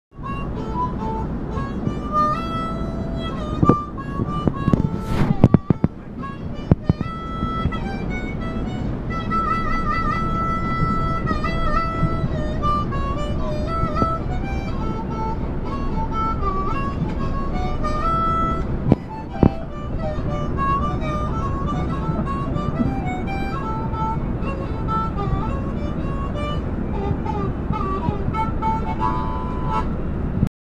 metro north harmonica train
who is this guy?? fantastic. best ride to Harrison i ever took.  BIGToken.com